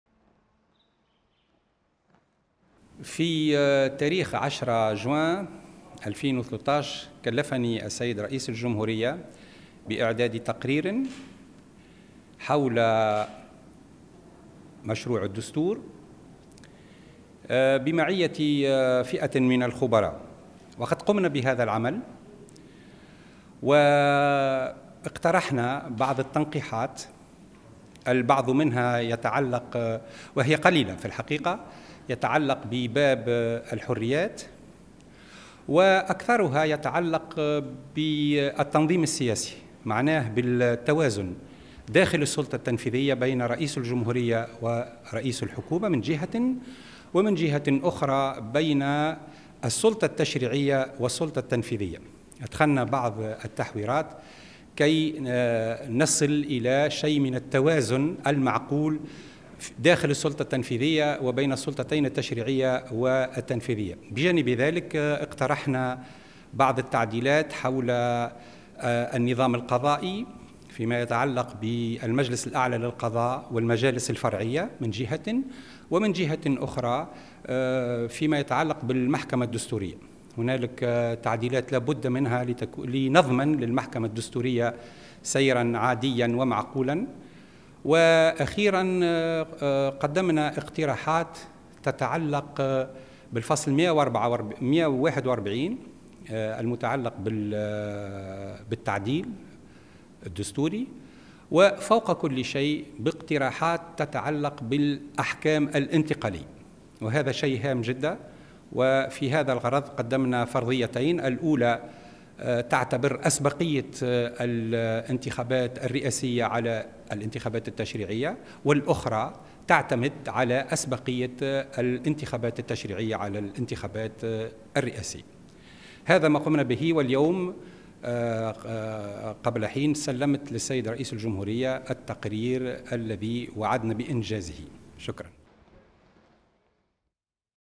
رئيس الجمهورية يتلقى تقريرا حول مسودة الدستور أعده عدد من خبراء القانون في تونس
تسلم رئيس الجمهورية السيد محمد المنصف المرزوقي صباح اليوم الثلاثاء من استاذ القانون عياض بن عاشور تقريرا أعده عدد من خبراء القانون في تونس حول مشروع مسودة الدستور وقراءتهم لمضمونه الحالي.
وعقب اللقاء، بين الأستاذ عياض بن عاشور أنه سلم رئيس الجمهورية تقريرا حول مضمون مسودة الدستور أعده عدد من خبراء القانون في تونس بناء على طلب منه منذ يوم 10 جوان الجاري، مشيرا إلى أن التقرير تضمن عددا من المقترحات والتوصيات المتعلقة في بعض جوانبها ببنود في باب الحريات فيما تعرضت بقية المقترحات إلى التنظيم السياسي وخاصة ضمان التوازن صلب السلطة التنفيذية بين رئيس الجمهورية ورئيس الحكومة من جهة وبين السلطة التشريعية والسلطة التنفيذية من جهة اخرى.
 و أضاف أن الخبراء اقترحوا إجراء بعض التحويرات في هذا الاتجاه لبلوغ توازنا معقولا"، وفق رأيه، بين طرفي السلطة التنفيذية وبين السلطة التشريعية والسلطة التنفيذية.
كما أوضح الأستاذ عياض بن عاشور أن التقرير تضمن في جانب اخر منه مقترحا بإدخال بعض التعديلات المتصلة بالنظام القضائي وبالمجلس الأعلى للقضاء والمجالس الفرعية المنبثقة عنه و بالمحكمة الدستورية داعيا إلى ضرورة إدخال بعض التنقيحات لكي تضمن المحكمة الدستورية سيرها العادي. ولفت في الاتجاه ذاته إلى أن مقترحات الخبراء طالت ايضا الفصل 141 من مشروع الدستور المتصل بالتعديل الدستوري و بالأحكام الانتقالية.
وأشار إلى أن التقرير تضمن قراءة الخبراء الذين ساهموا في إعداد التقرير لمسألة اسبقية تنظيم الانتخابات الرئاسية عن الانتخابات التشريعية أو تنظيم الانتخابات التشريعية قبل الانتخابات الرئاسية